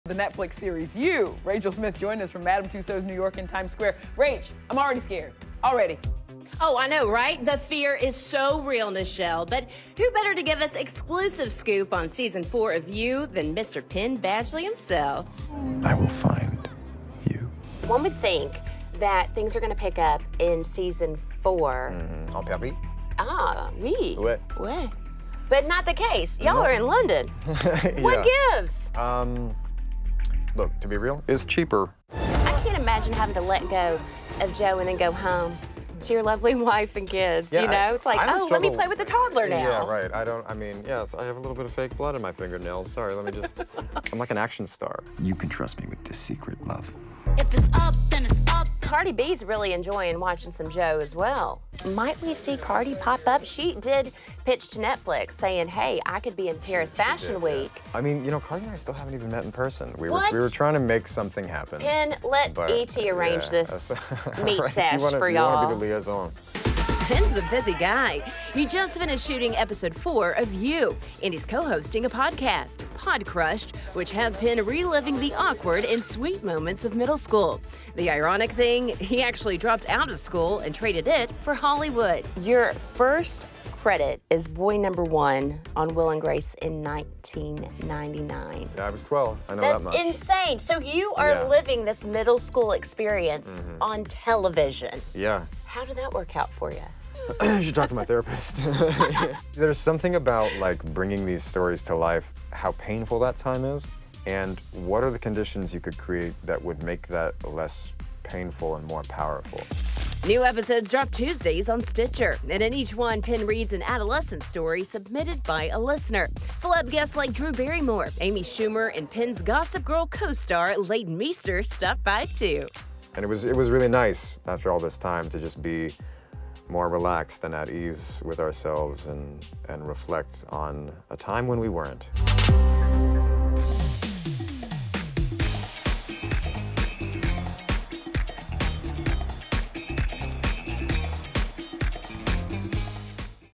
Penn Badgley on Where Season 4 of You Picks Up (Exclusive)
Penn Badgley chats with ET about ‘You’ season 4 and his podcast, ‘Podcrushed,’ which drops new episodes on Wednesdays on Stitcher.